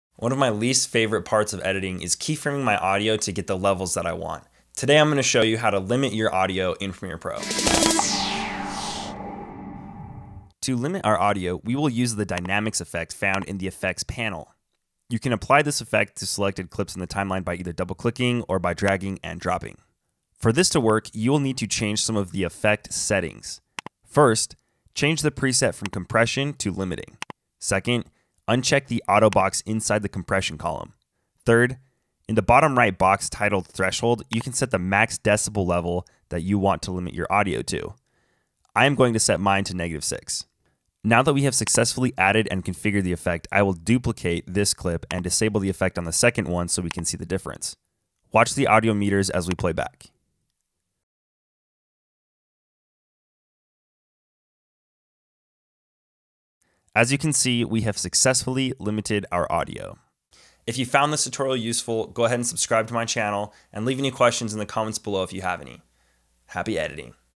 How to Limit Audio in Premiere Pro
Tired of key-framing all your audio levels? An audio limiter can make your life a little easier!